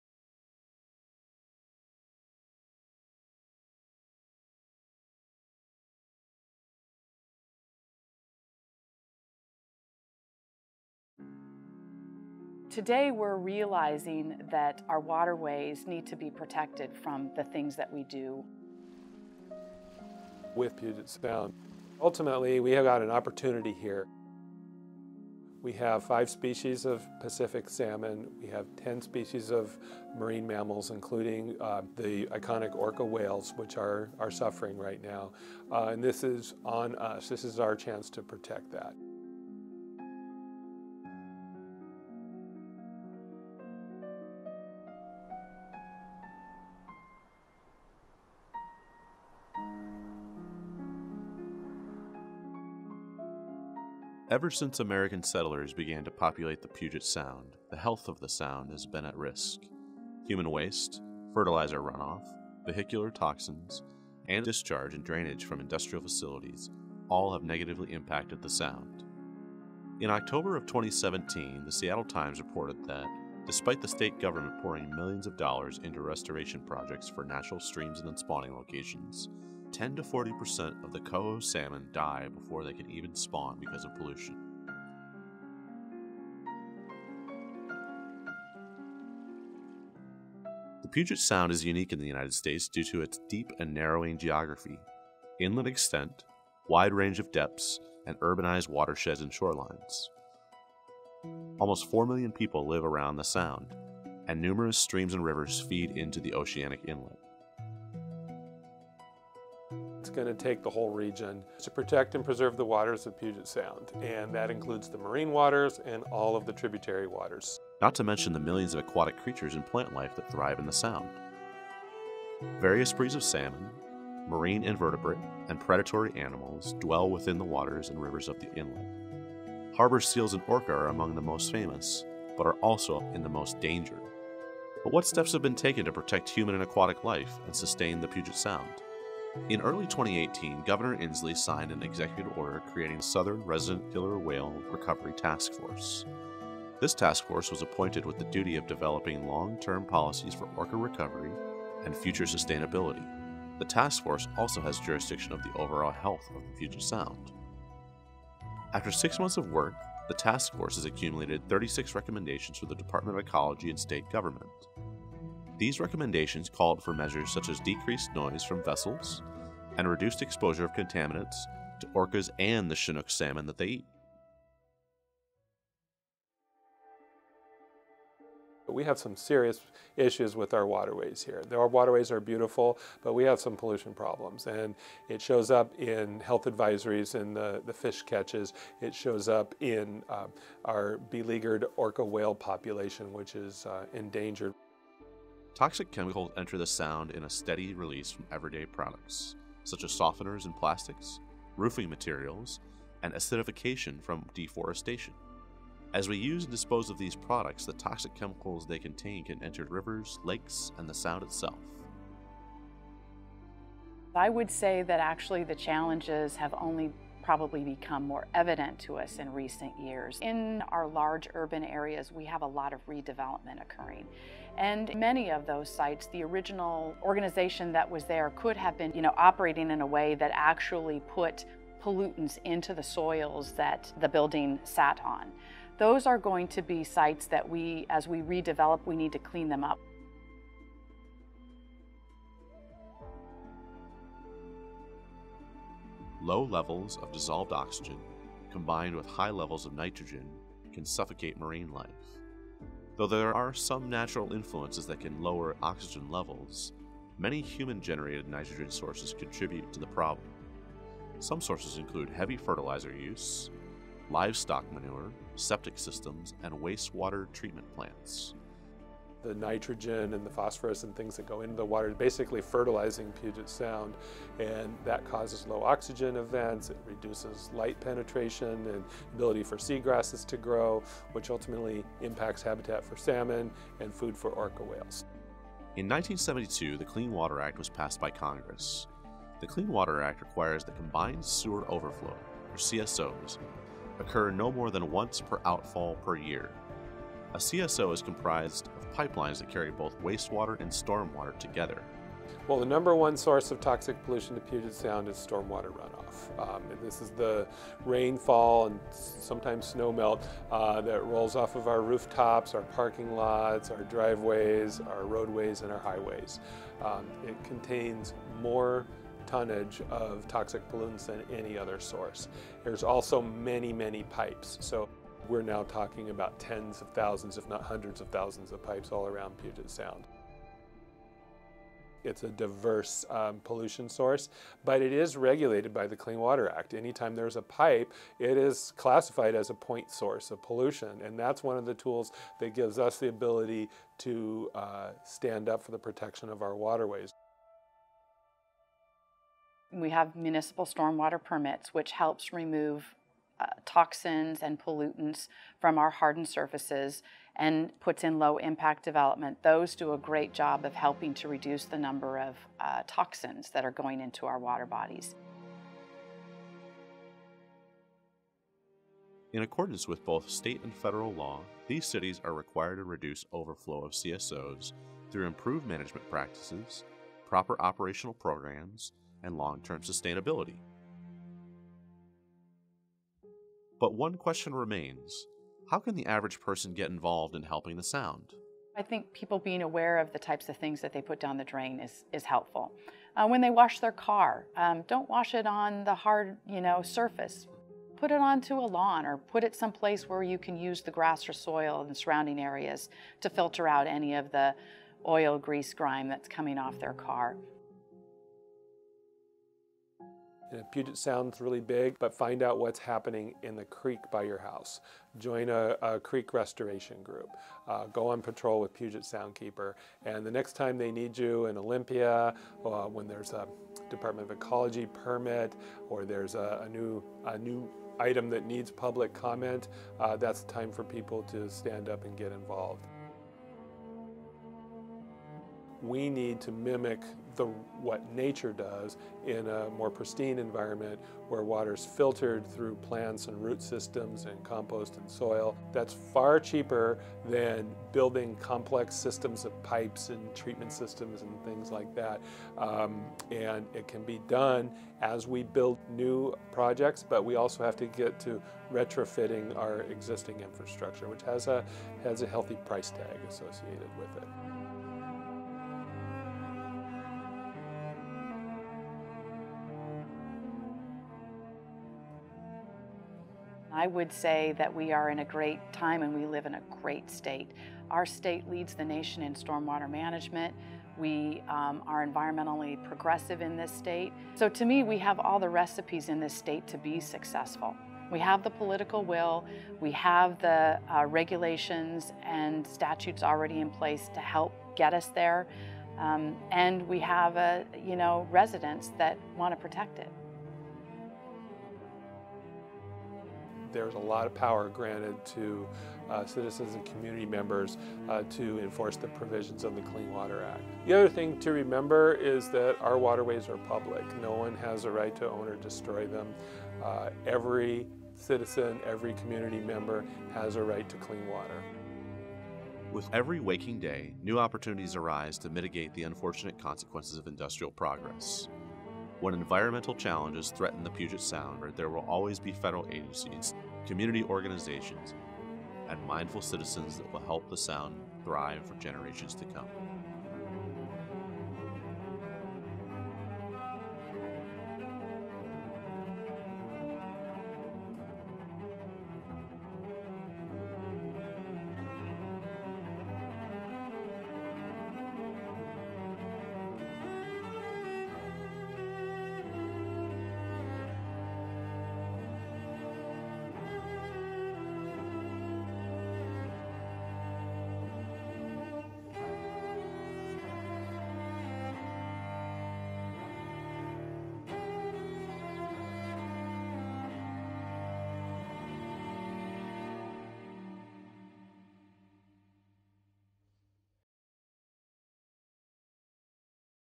Into Salish Grove - The Health and Safety of the Puget Sound
Nominated for an Emmy by the Northwest Chapter of the National Academy of Television Arts and Sciences (NATAS NW) 2019 Awards for Short Form Non-Fiction.
In this documentary, we covered the environmental problems that the Puget Sound faces, and what steps have been taking in the past and today to protect the Inlet.
I functioned as Director, Writer, Editor, Camera Operator, Interviewer, and Audio Engineer.
Produced at Bates Technical College.